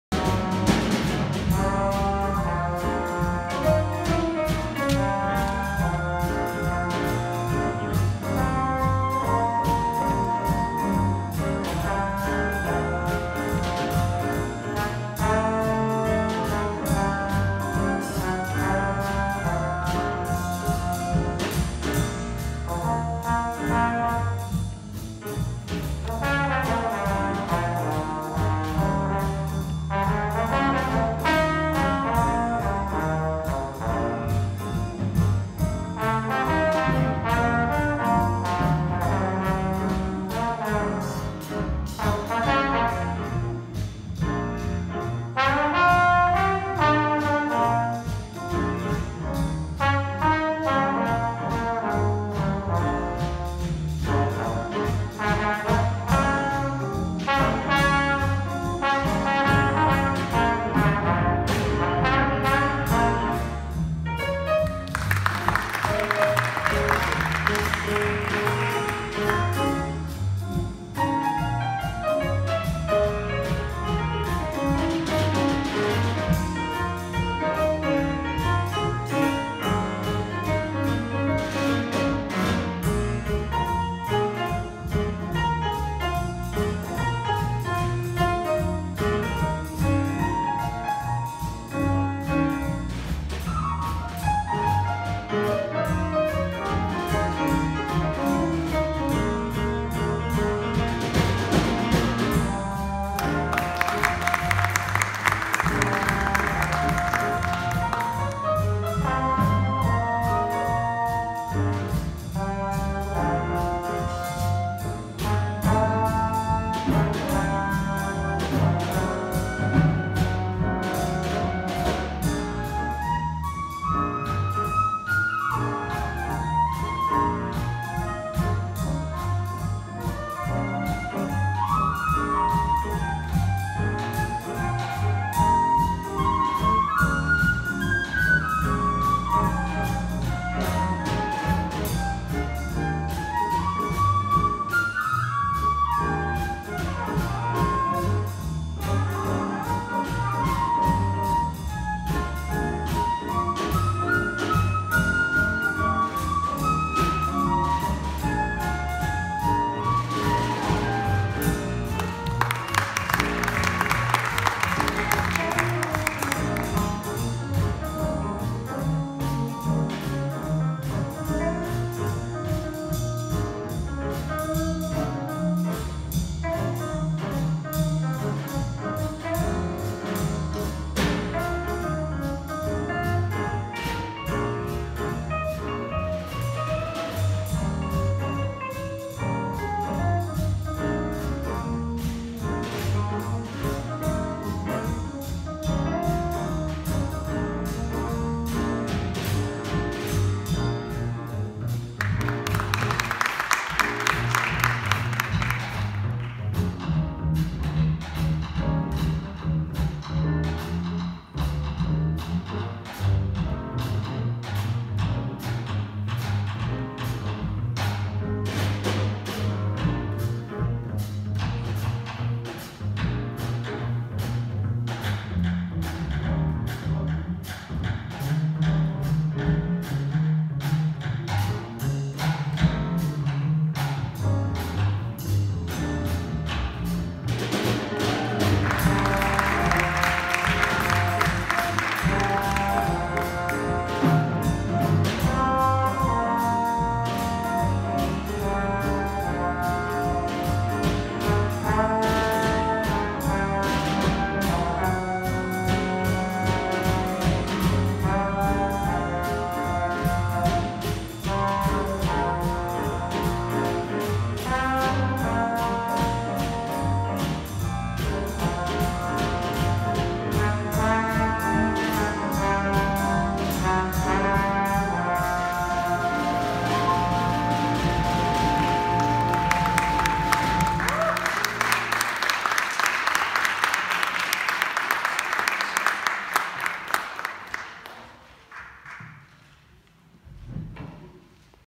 Jazz Night 2017 - UMass Boston
Jazz Night - University of Massachusetts Boston Students Jazz ensemble